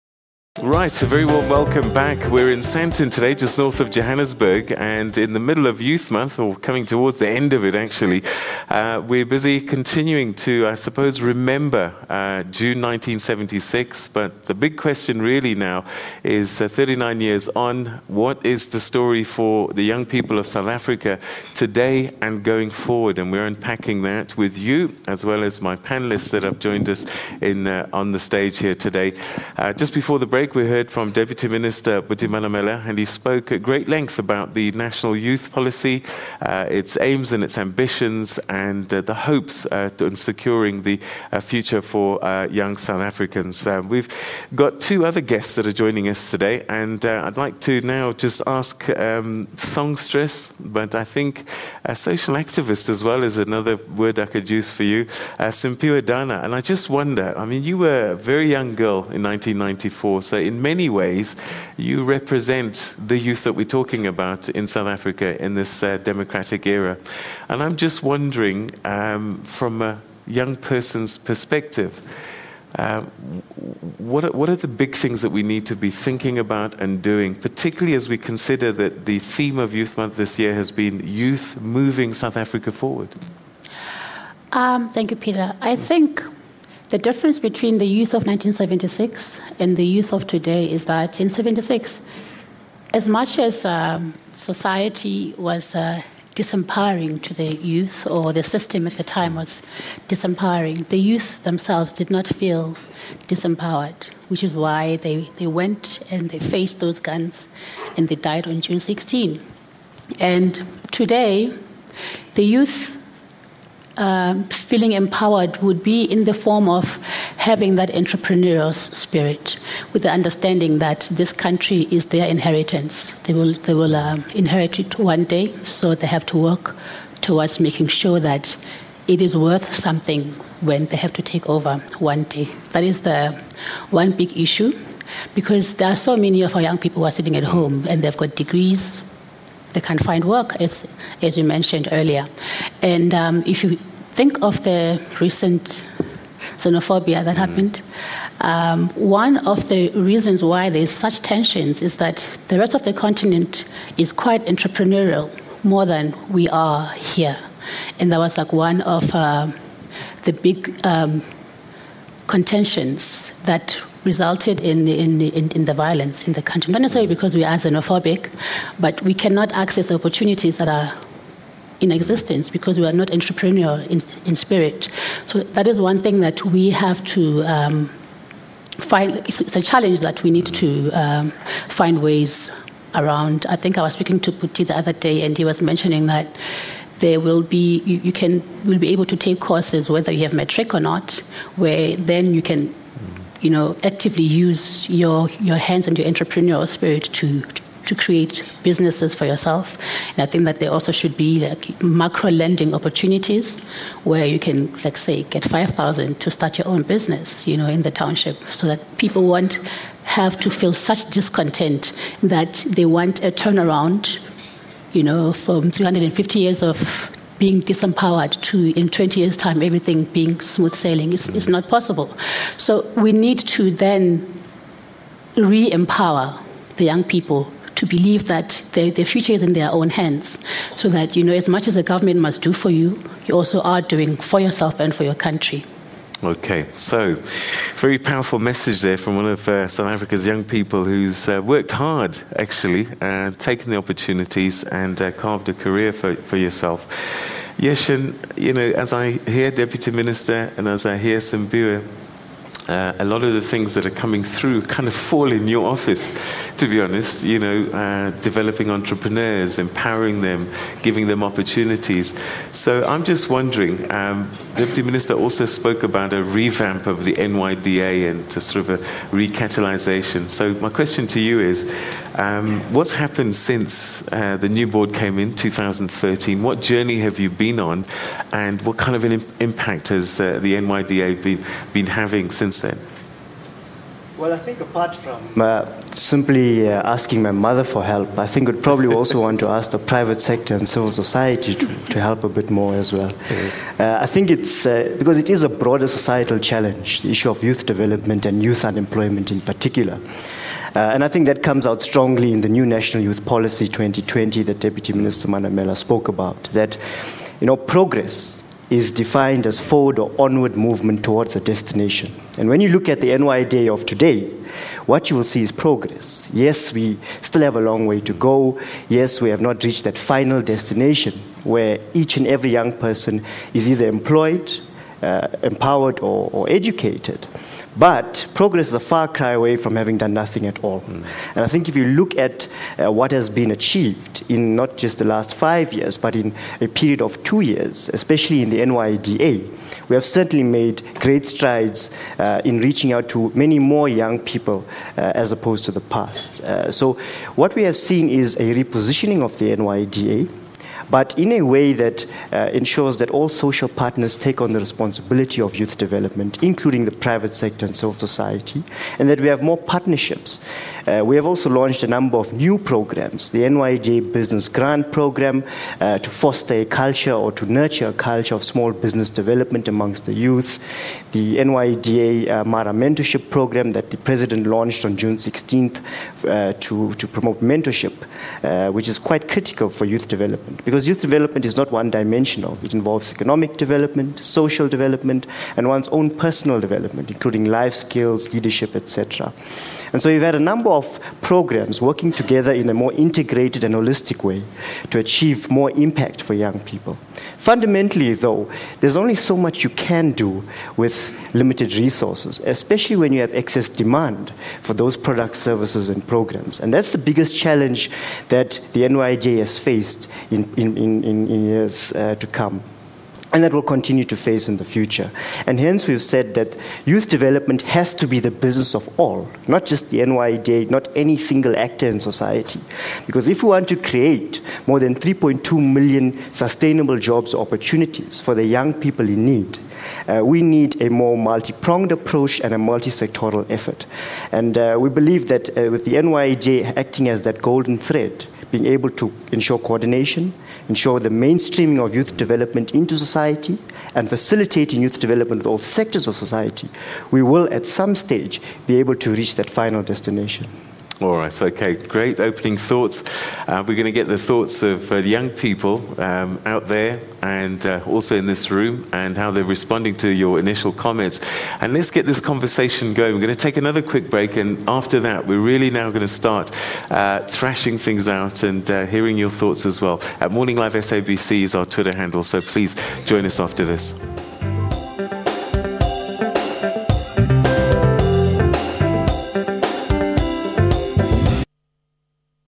We need to re-empower young people: Simphiwe Dana
Peter Ndoro at TNA in Sandton with Buti Manamela, the Deputy Minister for National Planning & Monitoring & Evaluation in the Presidency, Yershen Pillay, the Executive Chair of NYDA and Simphiwe Dana - Musician and an activist. The Theme: Youth Moving SA Forward...